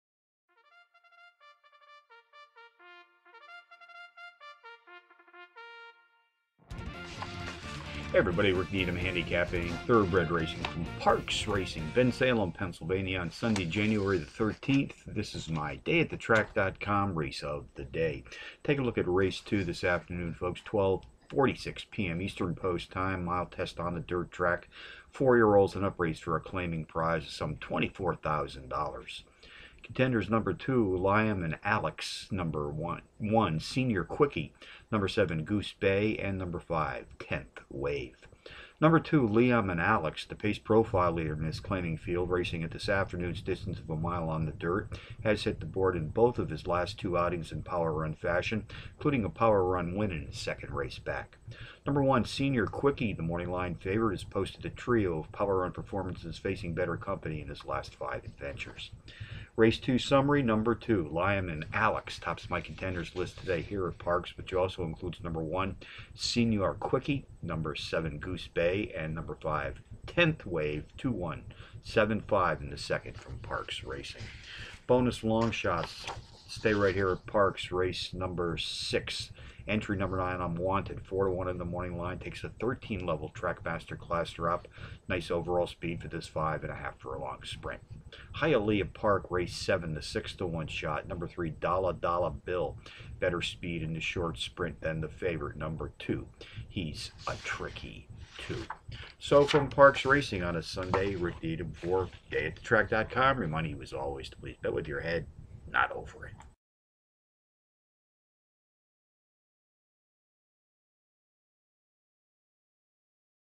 Race #2 - Parx - 1/13/2012
Race #2 - Parx Racing - Sunday, January 13, 2013 + bonus longshots